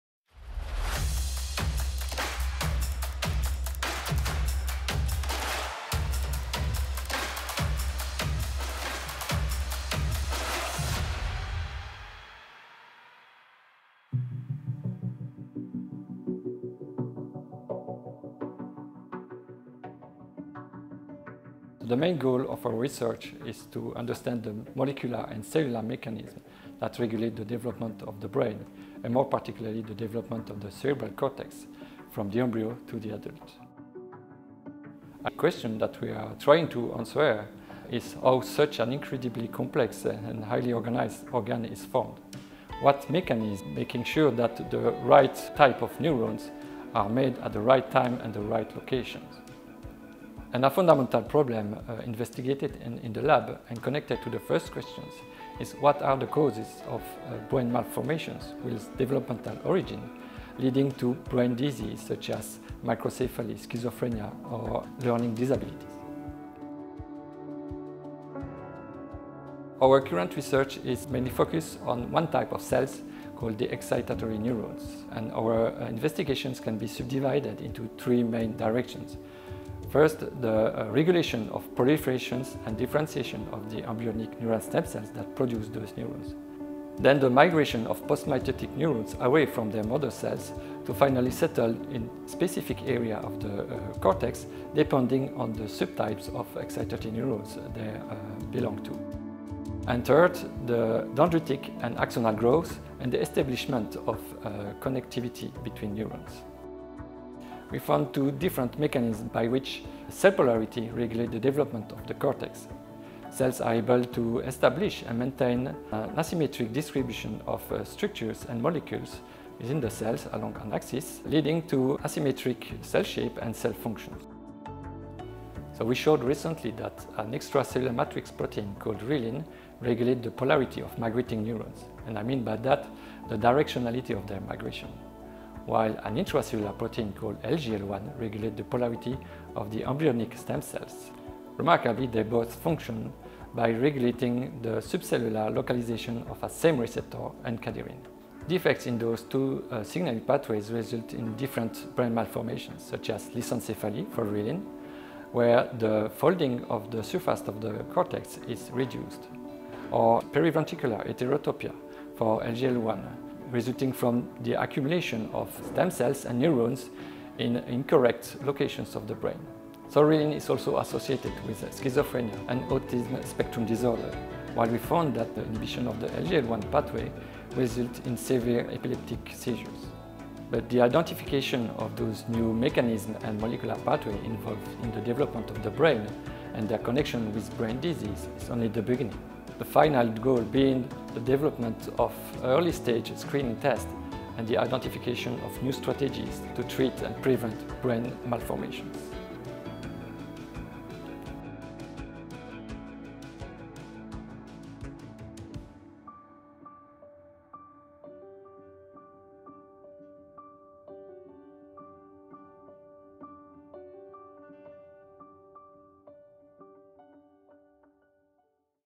Neurons: from scratch to cerebral cortex
The mammalian cerebral cortex plays key roles in learning and memory, the control of sensory and motor functions or of our emotions. The proper execution of these tasks depends on a long process of embryonic and postnatal development which relies on the production by neural stem cells of the appropriate number of neurons with proper identities that migrate to their specific location and make connections with each other. Defective signaling pathways during development are the causes of human brain diseases such as lissencephalies and heterotopias, dyslexia, schizophrenia, autism, epilepsy and many others. The goal of our laboratory is to better define the mechanisms of neural cell proliferation, migration and differentiation in order to understand how the cerebral cortex is built and learn how to treat and prevent devastating brain malformations.